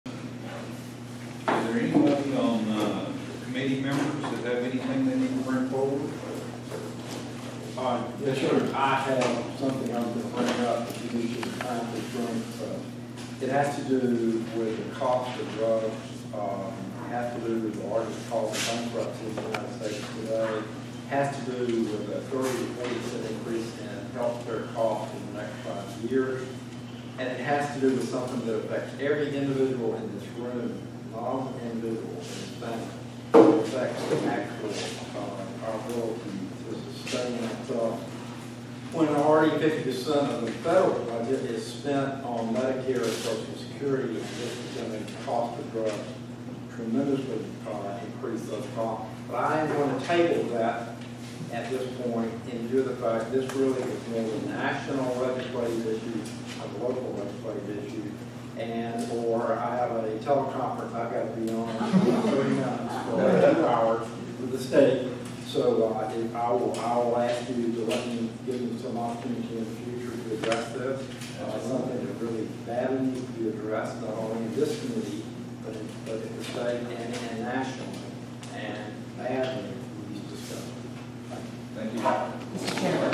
GAC Member Dr Grow - Health Department @ GAC Chamber Members - 2015-09-08
Valdosta Lowndes County Chamber of Commerce - Members to Be Heard - Governmental Affairs Committee) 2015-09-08